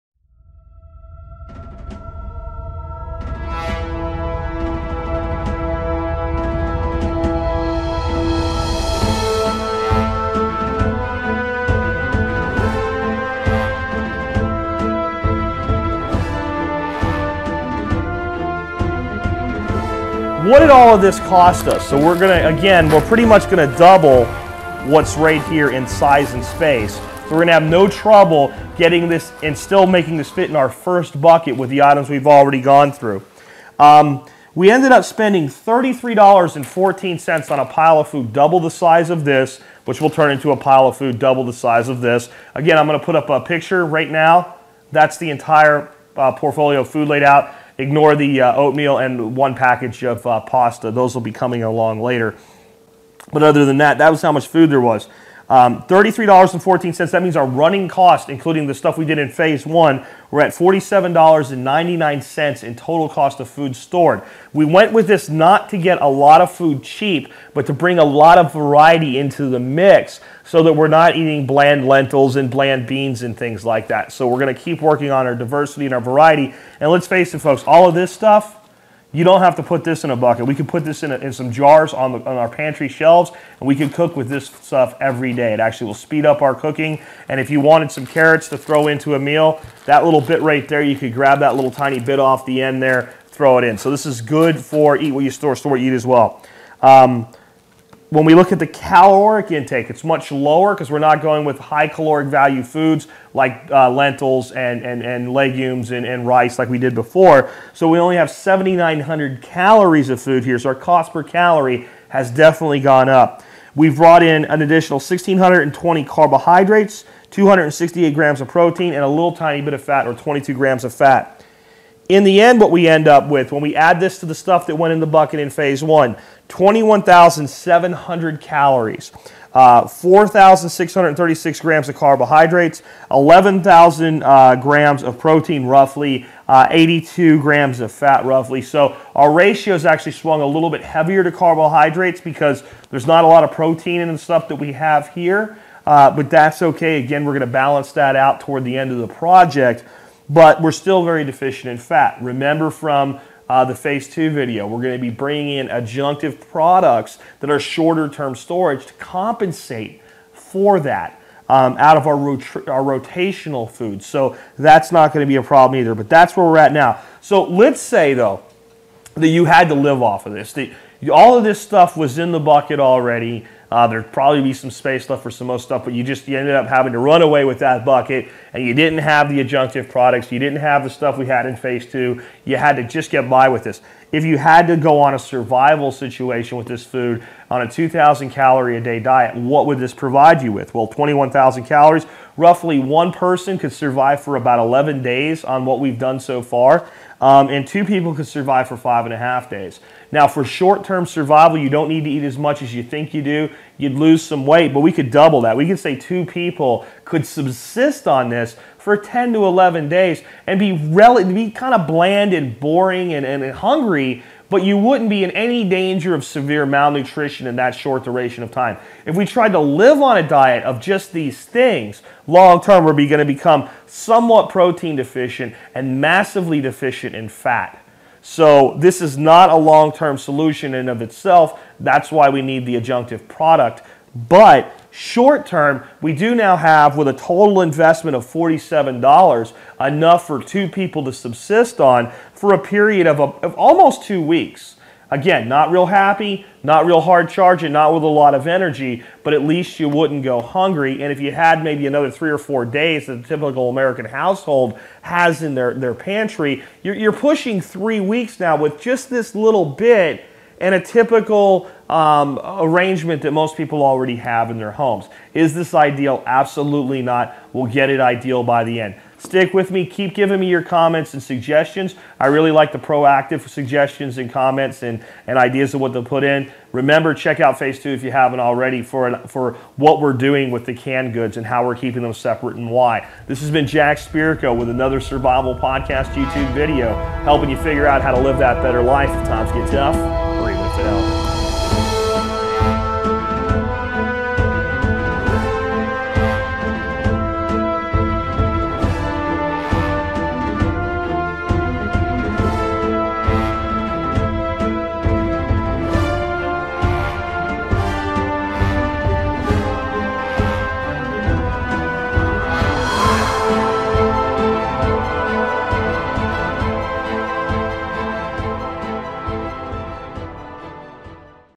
The 5 Gallon Bucket Food Storage Project - Part 3-B
This is the conclusion of the third phase of the long term food storage project with 5 gallon buckets.  Today we add dehydrated vegetables to the mix.  All the vegetables in this video were dehydrated with an 9-tray Excalibur dehydrator.